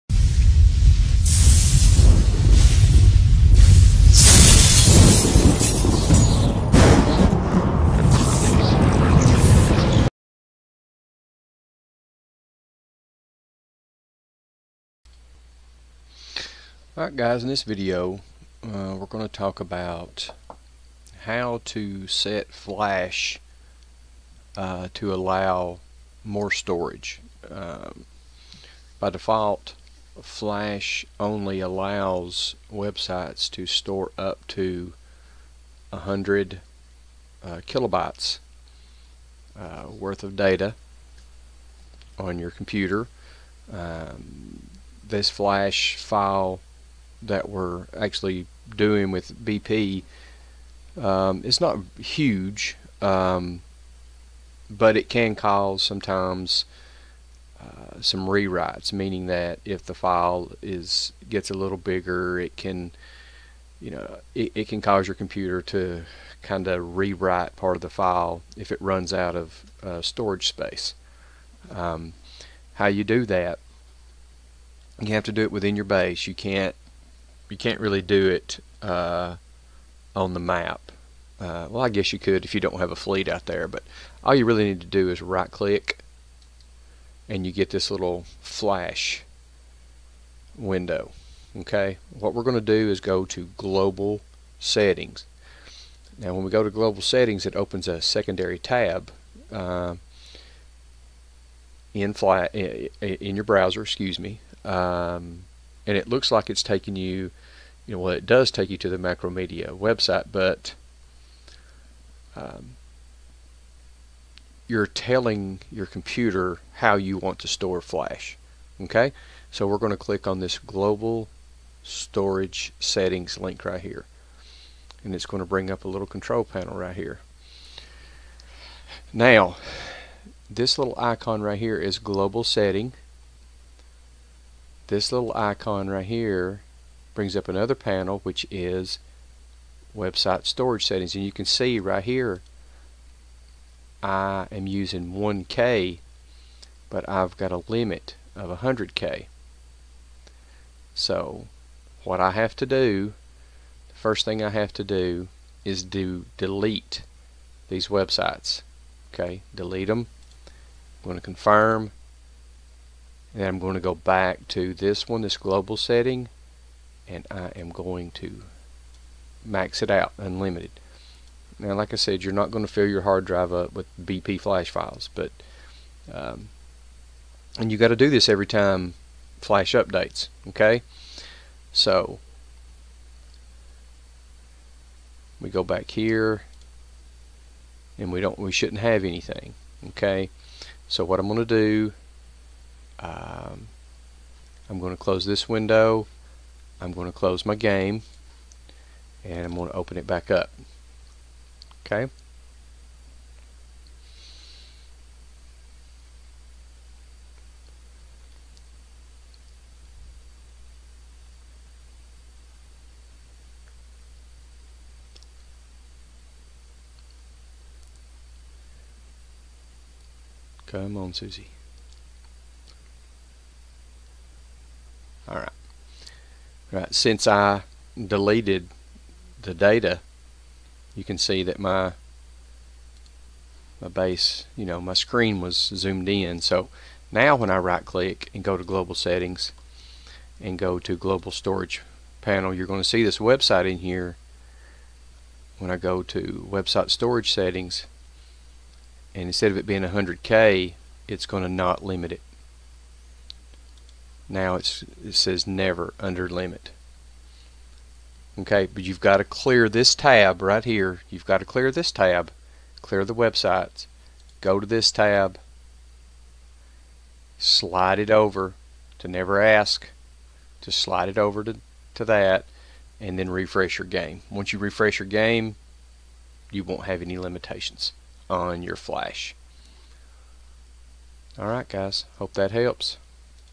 How to change your Flash Storage Settings
Battle Pirates stores data on your PC.  This video will show you how to allocate more room for the game on your hard drive.